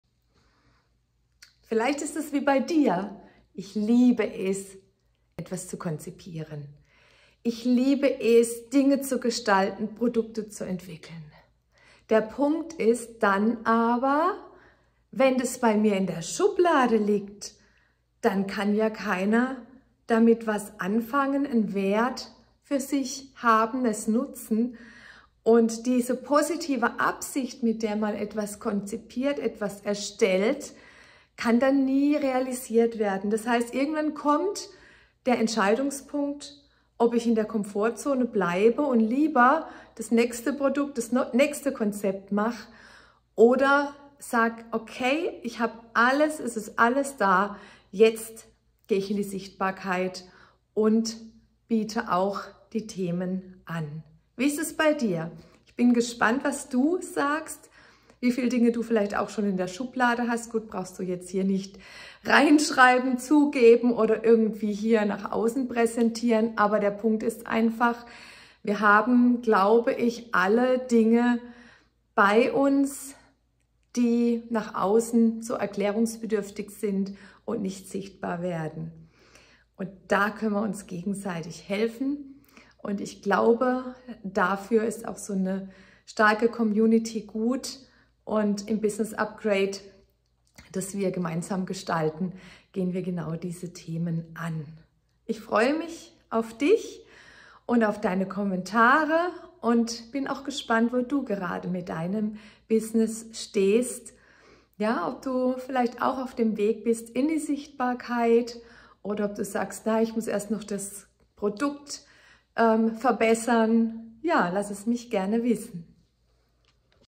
Sichtbarkeit mit dem BusinessUpgrade der INITIAL-Akademie
Wertvolle Produkte, Online-Kurse oder fertige Seminare liegen in der Schublade? Sichtbarkeit ist ein Schritt aus der Komfortzone. Wenn ich zurück denke, dann frage ich mich manchmal, wann das eigentlich ein Ende hatte.

Jahrelang war ich gestresst vom Thema Verkauf. Ich hatte das Gefühl, nicht ich selbst sein zu können. Manchmal klingelte das Telefon im Büro. Am liebsten war es mir, wenn meine Mitarbeiterin einfach kurz das Angebot machte und den Interessenten zum Kunden für uns gewann.

Interessant, dass ich es heute liebe mit Interessenten zu sprechen. Es funktioniert, weil ich für mich eine Entscheidung getroffen habe. Es war die beste Entscheidung in meinem Leben. Es ist mir jetzt bewusst, wie viel Businessentwicklung mit Persönlichkeitsentwicklung und dem eigenen Mindset zu tun hat. Das Business darf leicht sein, sich richtig anfühlen und Freude machen.

Mut braucht es schon lange nicht mehr, dafür viel Empathie und das Wissen bereits im ersten Kontakt Mehrwert bieten zu können. Bammel vor einem Termin habe ich schon lange nicht mehr, dafür eher hinterher das Gefühl, dass es schade gewesen wäre, den Menschen nicht zu treffen.
Es gibt mir Erfüllung und das begeistert mich. Genau das wünsche ich dir auch.